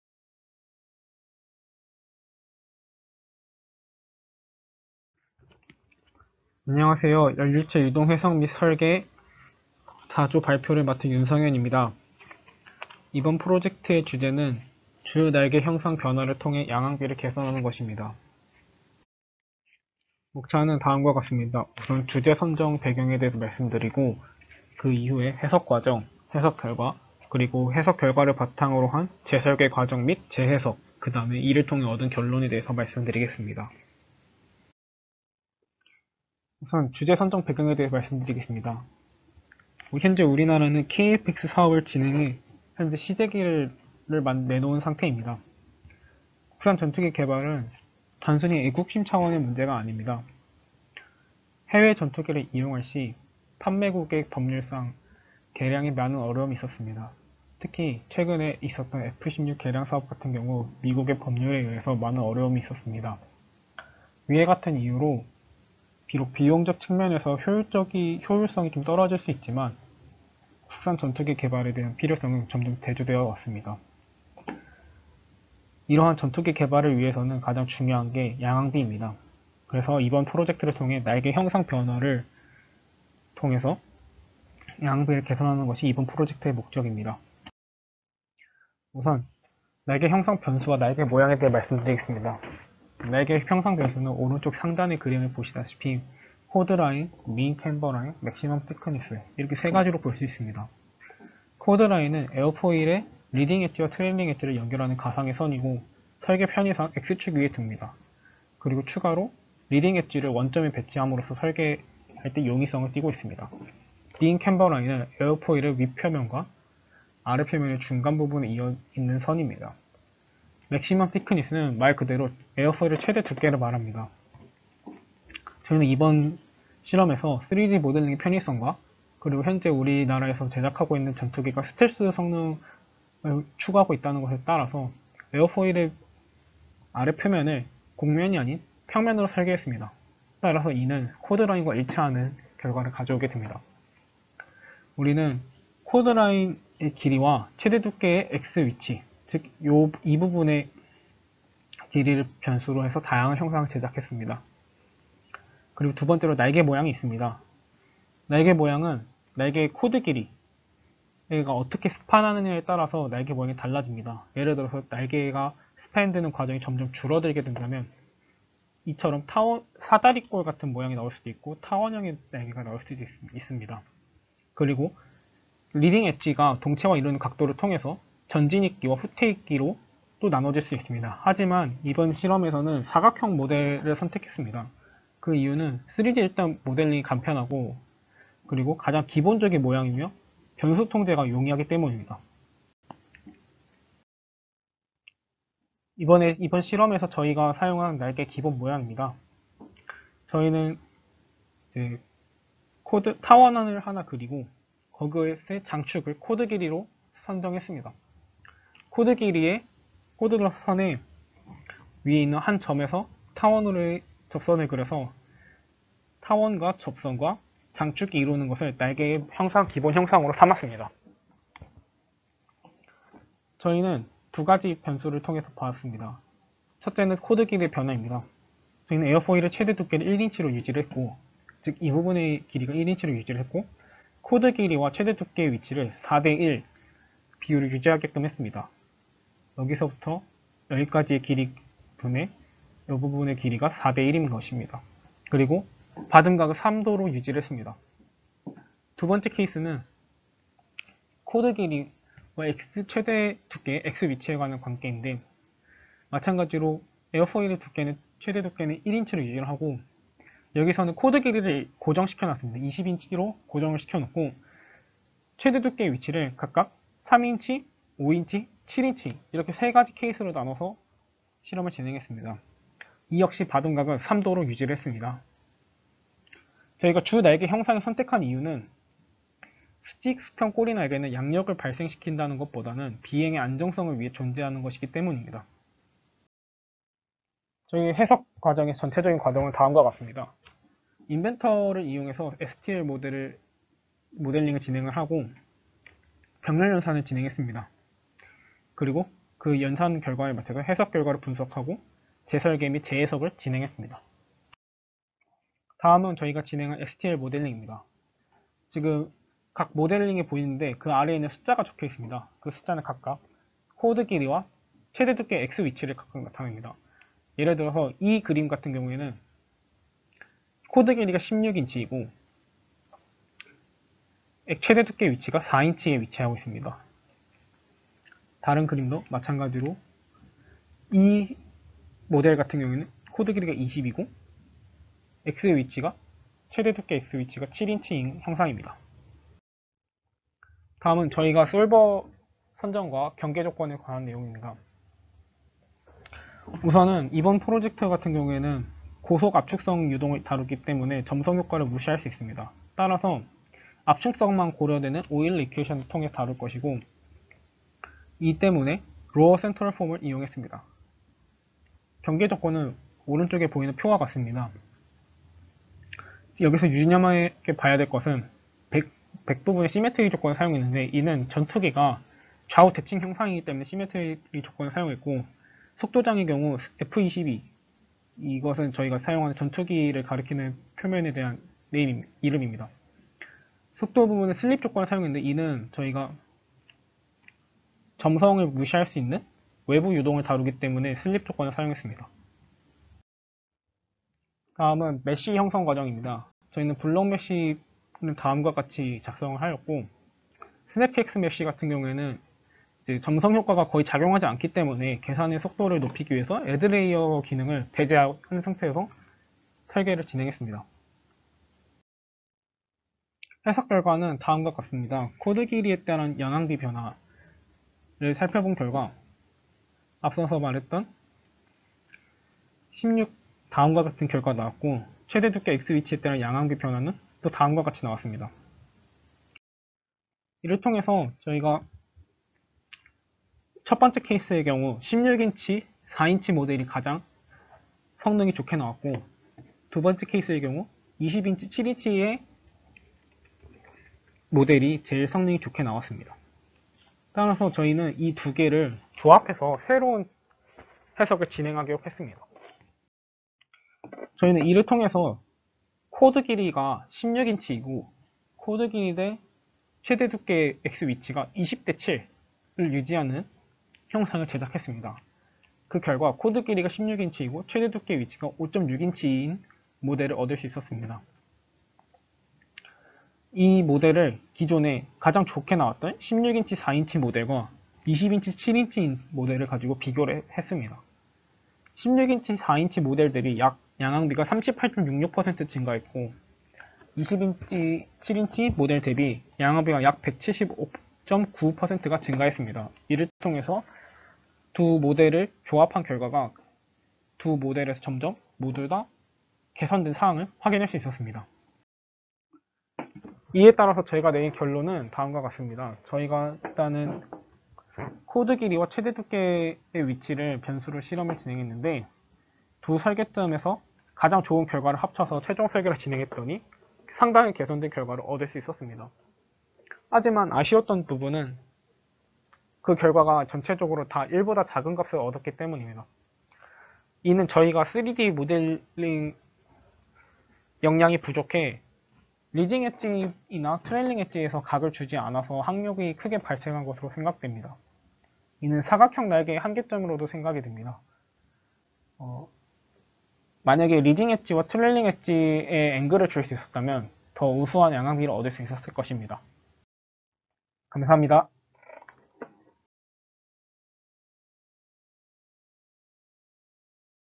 [우수상] 초음속 제트기 양력비 향상을 위한 주날개 형상 변화에 관한 수치해석적 연구 by.박재원, 윤성현, 표기열
2021 한양대학교 기계공학부 열유체유동해석및설계 All-Year Design Festival